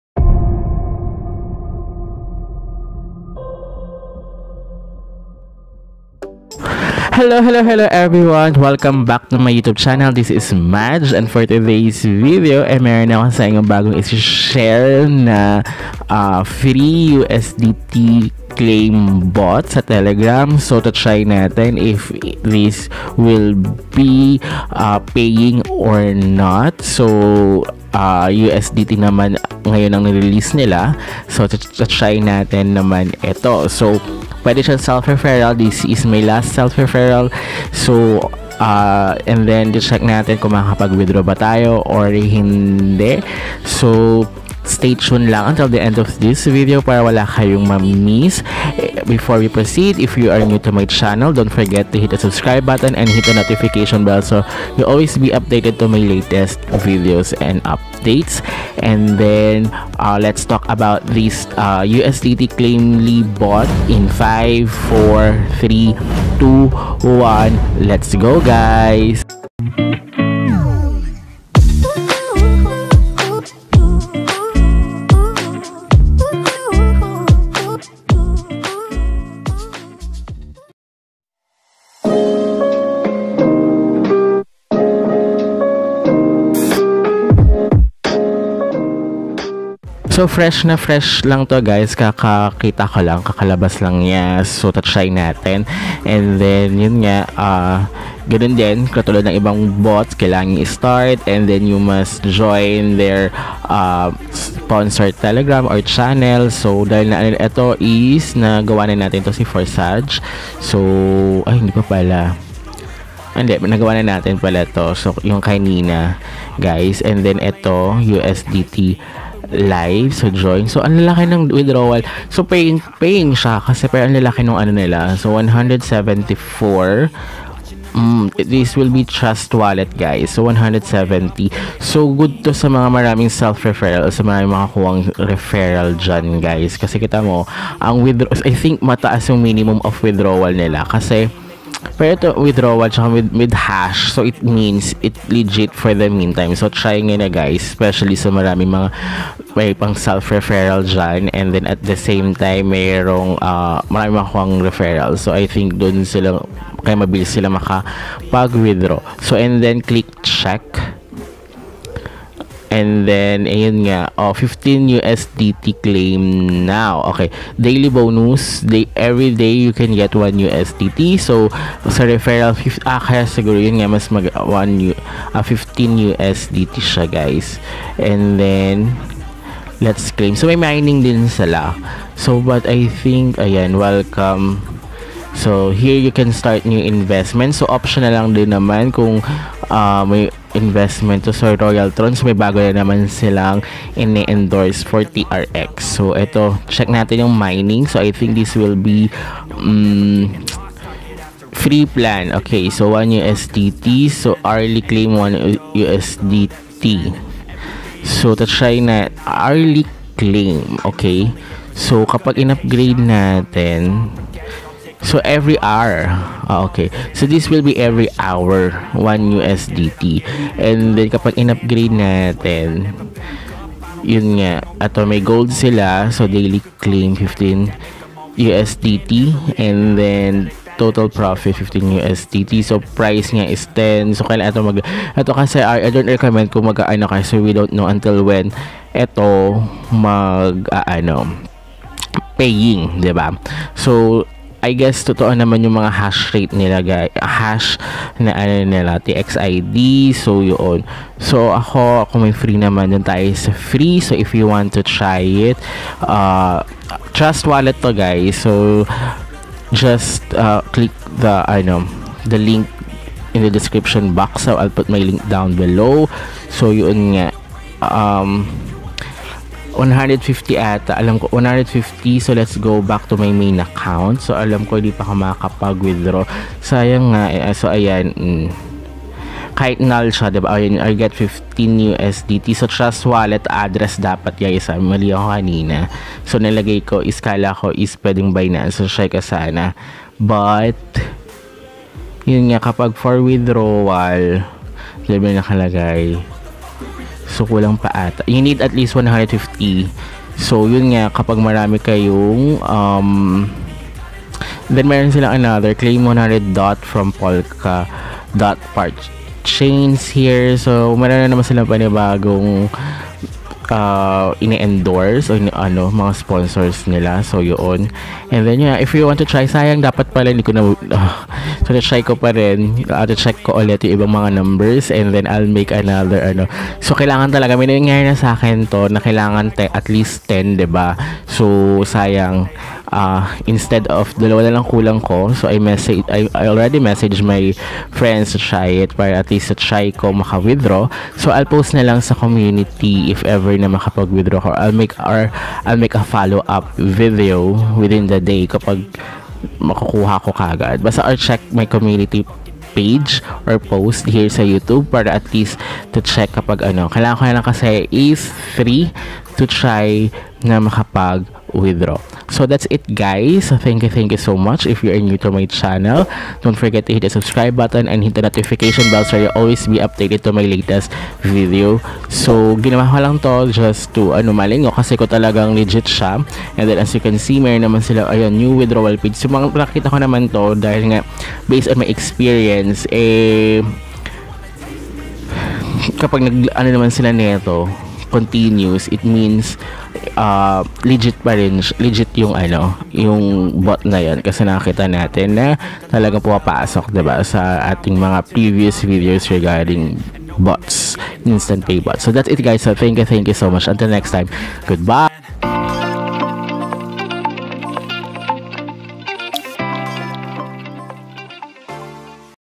NEW EARN FREE TETHER TOKEN USING TELEGRAM | CLAIM 1 USDT + SELF REFERRAL TRICKS| OPTIONAL INVESTMENT
NEW EARN FREE USDT TOKEN USING TELEGRAM | CLAIM 1 USDT + SELF REFERRAL TRICKS| OPTIONAL INVESTMENT

✅LEGIT FAUCET PAY CLAIM SATOSHI

✅TRADING PLATFORM LEGIT

Please send also the screenshot of your YOUTUBE profile also na nakaSUBSCRIBE ka para masama kita sa aking Congratulatory for may Next Video 😉

-The video will serve as a guide
-Don't Invest so easily because I Don't own the app or website because  it is possible the site is scam or it is not paying already.
- 👍Make sure you do your own research before you invest.
- invest only what you can afford to lose.

please also check my shop @ shopee.ph/madjluxuries